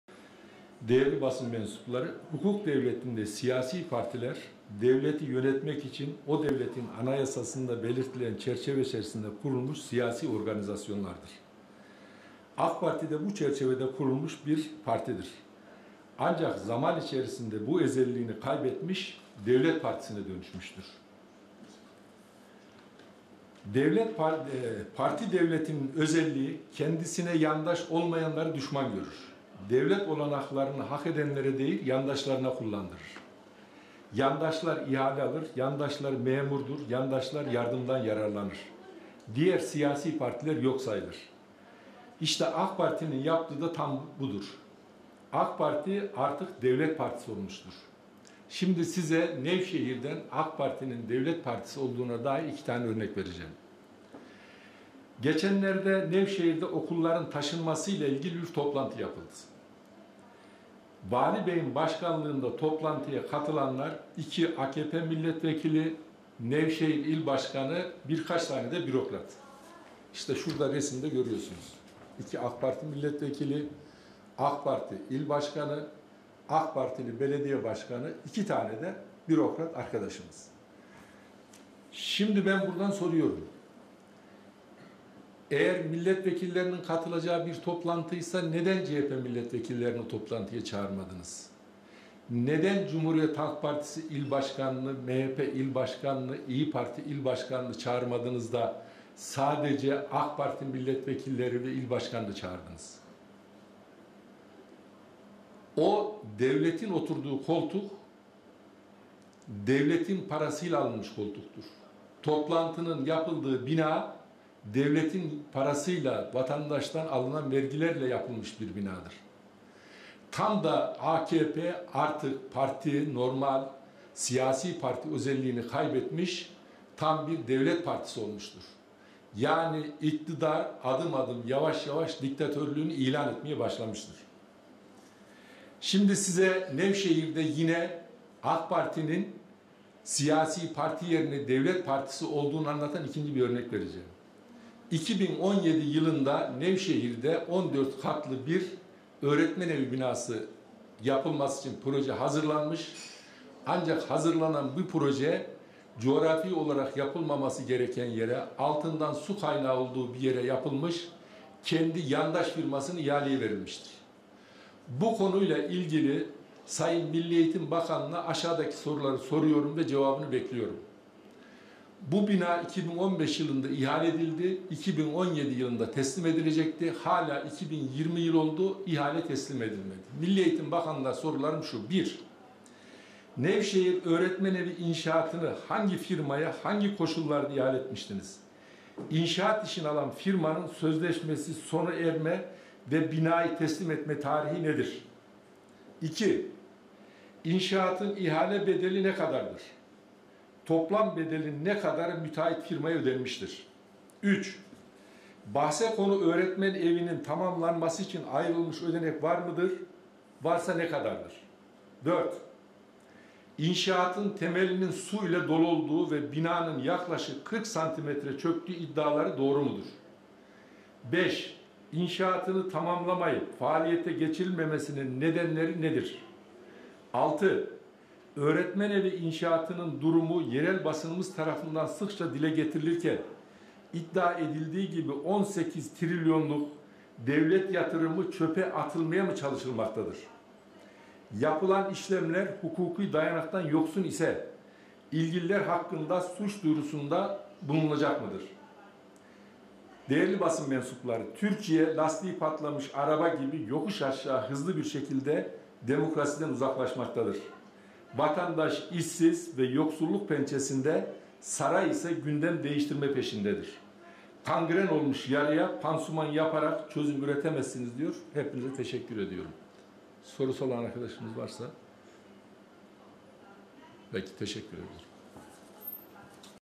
T.B.M.M. Basın Açıklaması..
Nevşehir Valiliğinde yapılan toplantı ve Öğretmen Evi İnşaatı ile ilgili T.B.M.M.'den yaptığımız basın açıklaması...